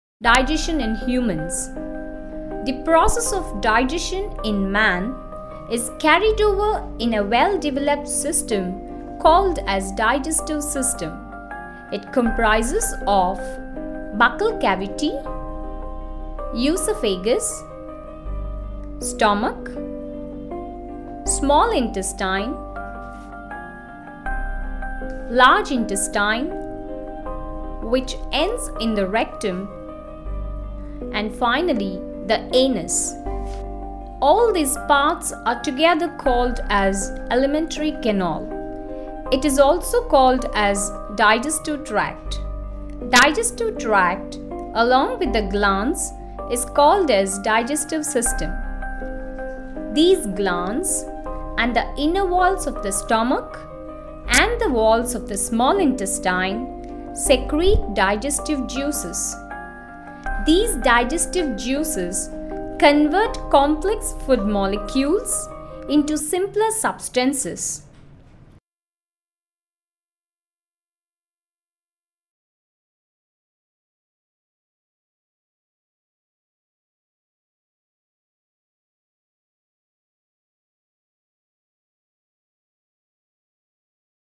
TECHNO Science class 6 - ORGAN SYSTEMS IN ANIMALS DIGESTIVE SYSTEM STRUCTURE AND PROCESS OF DIGES
TECHNO Science class 6

ORGAN SYSTEMS IN ANIMALS DIGESTIVE SYSTEM STRUCTURE AND PROCESS OF DIGES